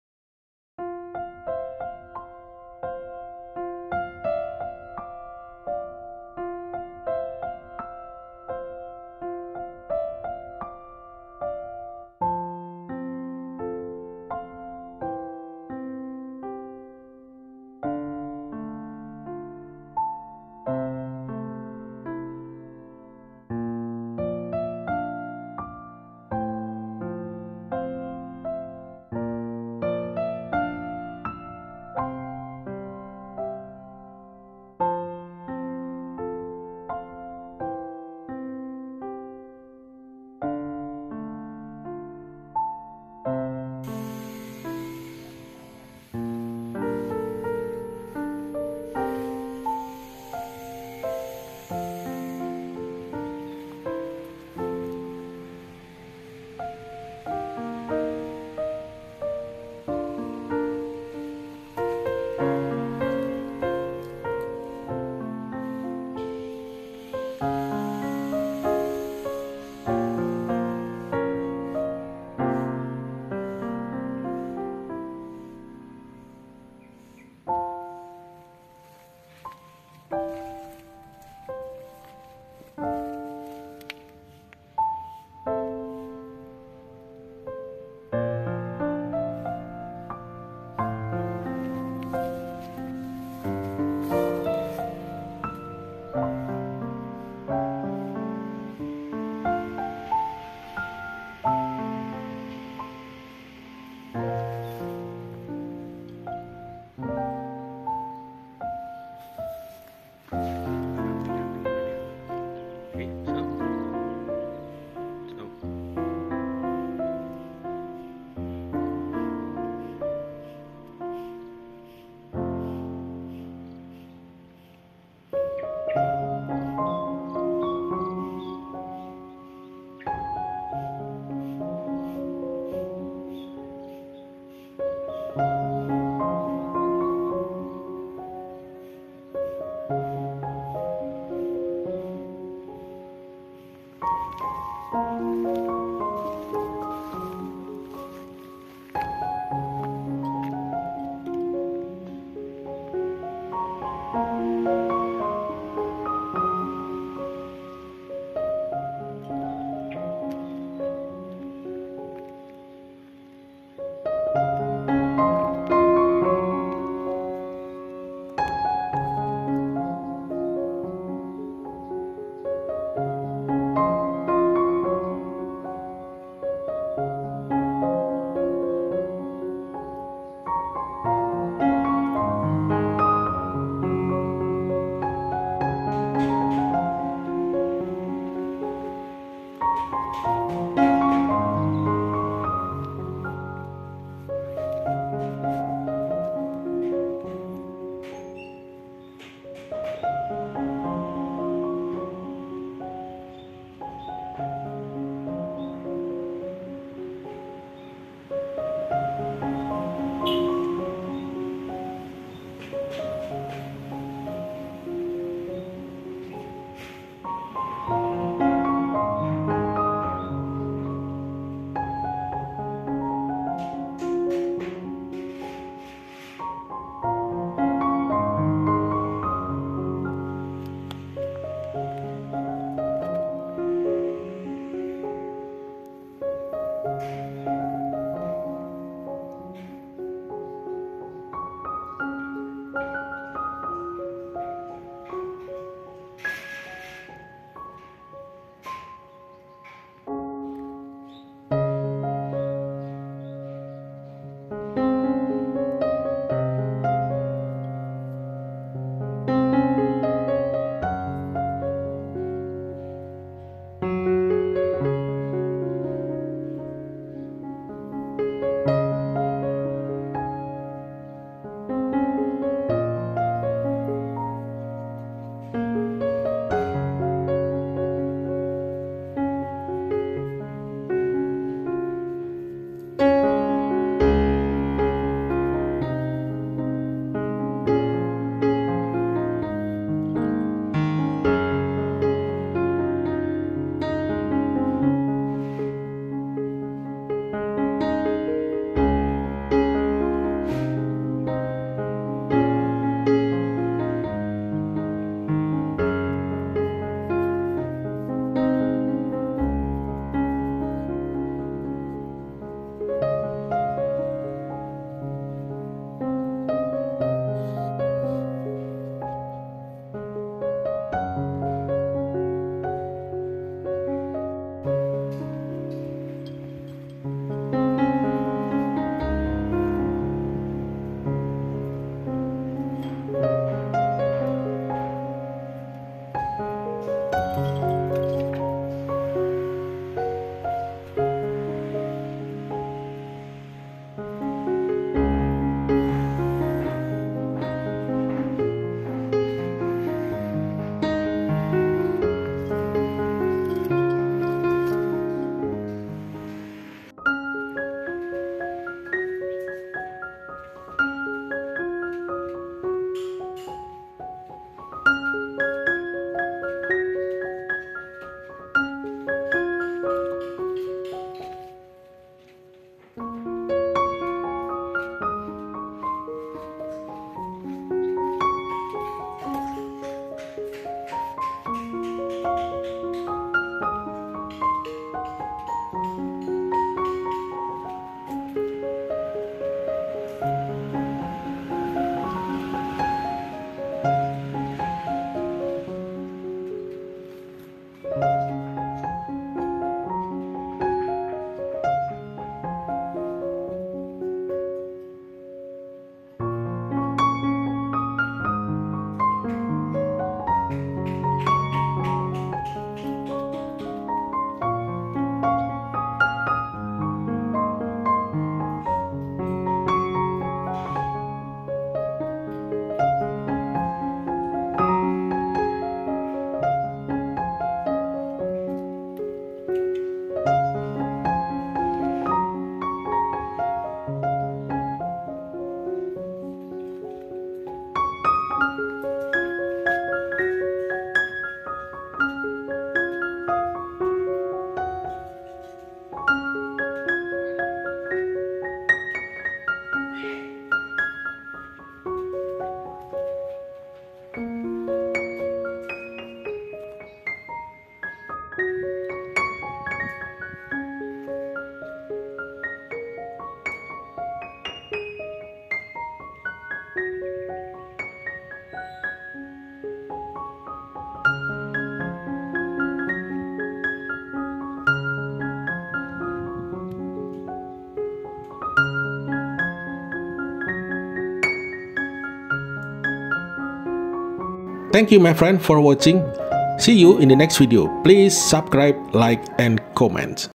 A mother cat who looks for something when the kitten is bored
Hello Cat Lovers, meet you again on Khozkha Meow Channel